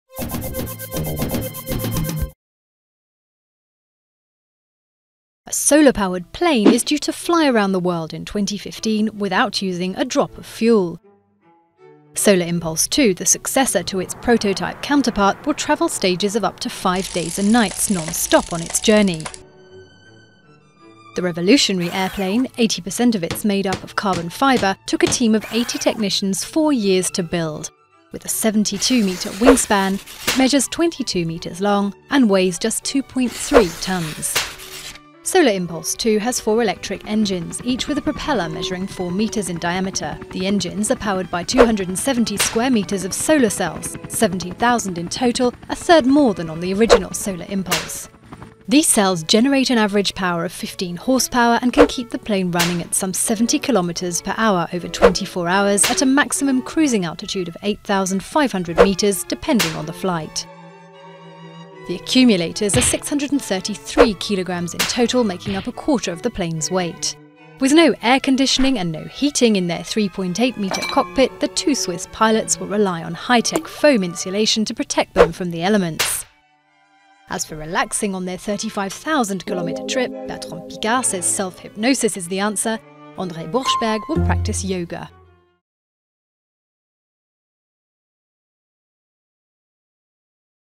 Solar Impulse 2
One of the masterminds behind a sun-powered plane makes his debut flight in the aircraft ahead of a planned round-the-world trip next year. Swiss pilot Andre Borschberg takes to the skies in Solar Impulse 2 on Friday 5 September. The airplane is the successor of an original plane of the same name, which last year completed a trip across the United States without using a drop of fuel.VIDEOGRAPHIC